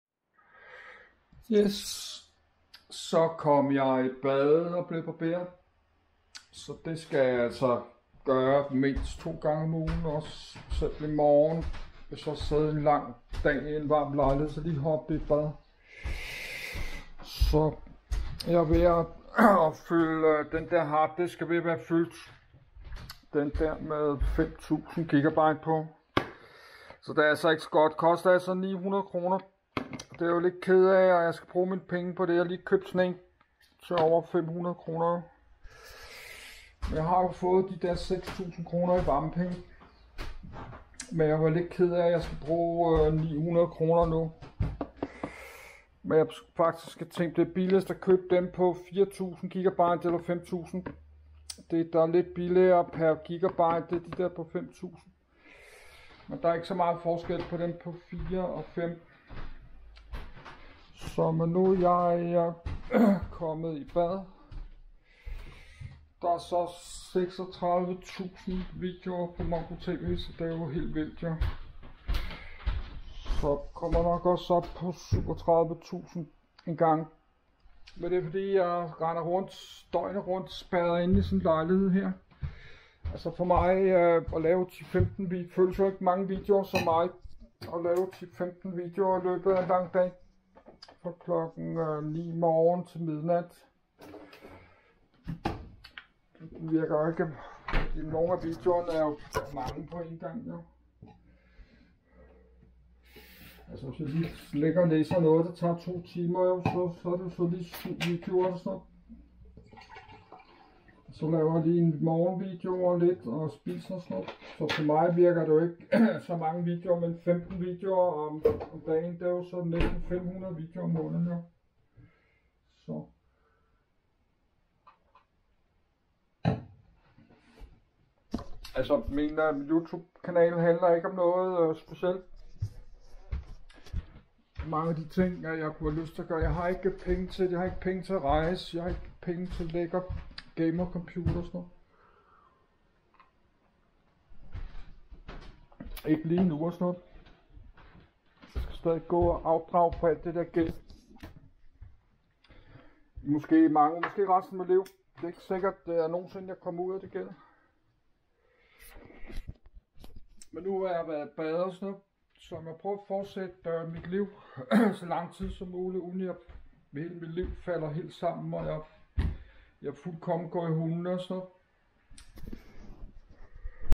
MongoTV_12429 - BAD og BARBERING På En Varm og Lummer TIRSDAG Sommer 2023 Formiddag
God TIRSDAG. Så kom jeg i bad, og blev barberet. Det må jeg få gjort altid, omkring 2 gange om ugen, og gerne 3 gange. Når det er varmt og lummert som nu, hele sommer, så kunne det også være en god ide, at jeg lige går i et hurtigt sent aftenbad, før jeg sover og går til ro. Der kan blive lidt over 30 grader varmt og lummert i min stue, hele dagen, og 25-26 grader hele natten. Det var varmt og lummert i nat igen i min stue, så jeg havde svært ved, at falde i søvn, og sov først, omkring klokken 4 nat.